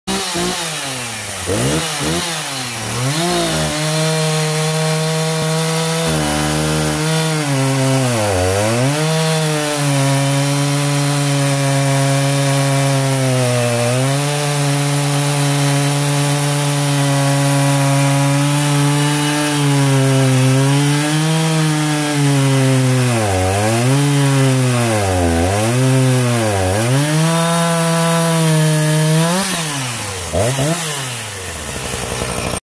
Stock Husqvarna 359 chainsaw 36" bar
This is a before video of it cutting very hard oak. It was tore down for a cylinder port job afterwards. The after video will uploaded next.
This isn't meant to highlight the stock 359, it's only to document the difference in performance between this saw stock versus ported. The 36" bar set up was how it was presented to me. Personally I don't run bars that long on my saws. This is however a good test to see exactly what kind of performance gains it achieved with the bar that long since that thing easily maxed out the saw.